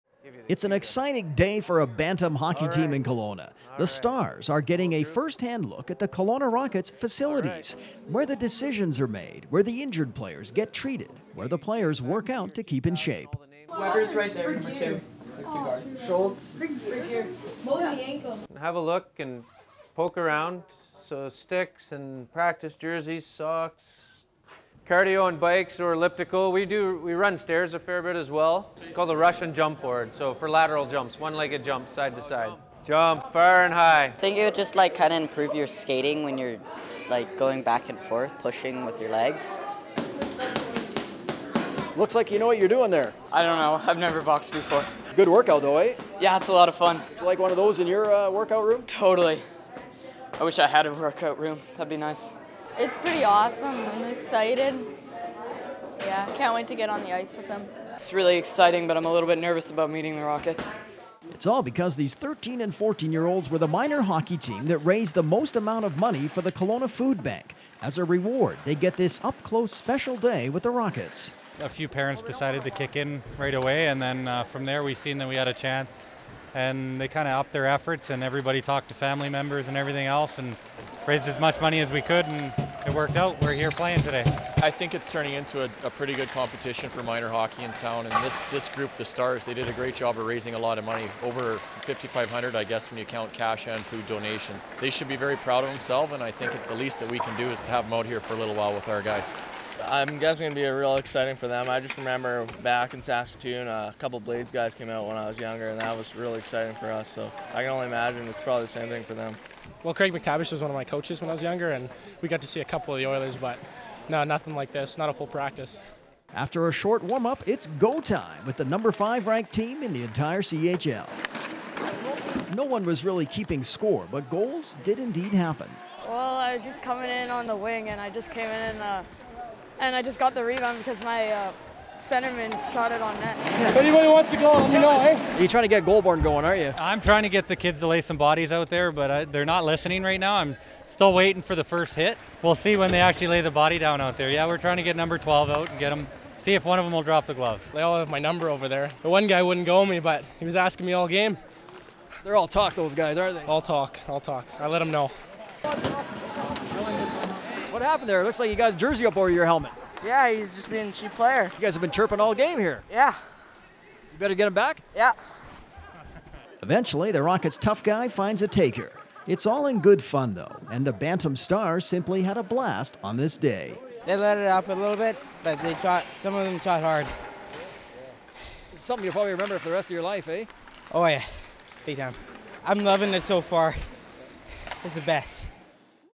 Day with the Rockets
Minor hockey players get a tour of the Kelowna Rockets facilities, and lace 'em up and scrimmage with one of the top teams in the Western Hockey league.